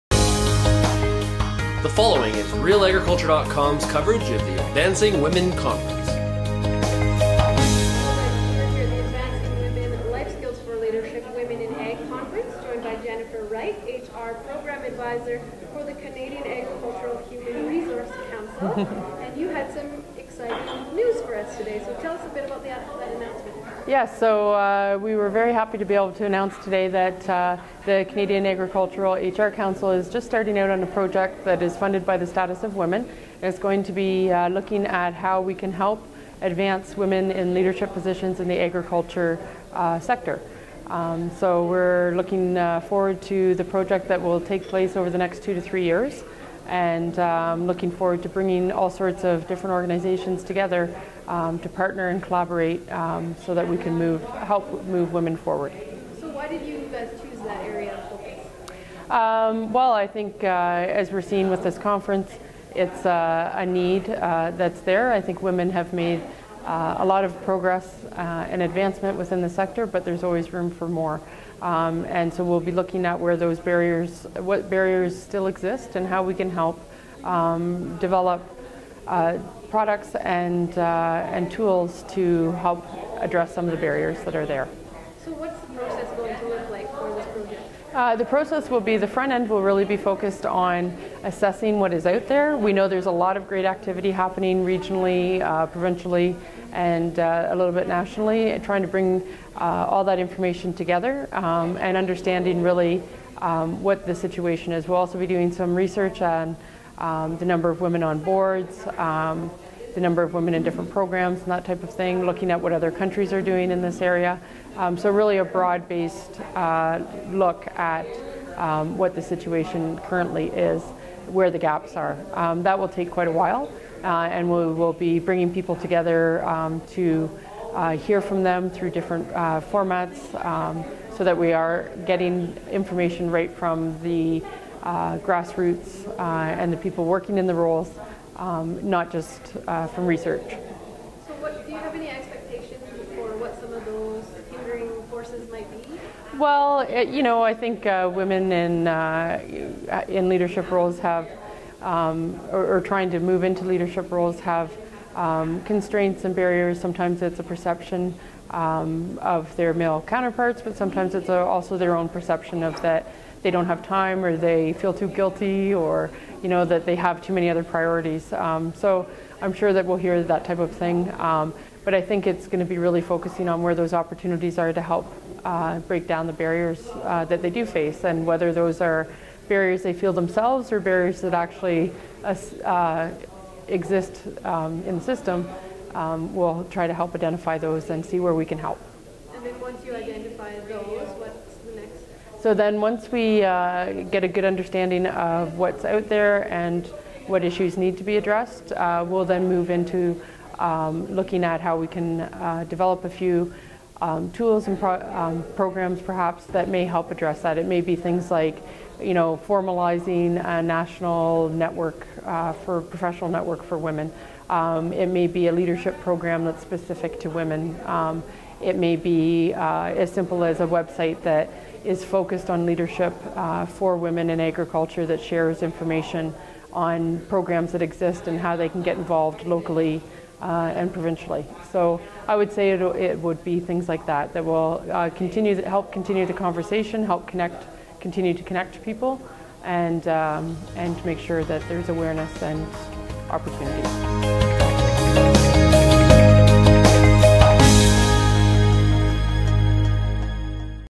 New Initiative to Study Barriers to Women's Advancement in Agriculture
Jennifer Wright speaks on the new initiative: Supporting the Advancement of Women in Agriculture.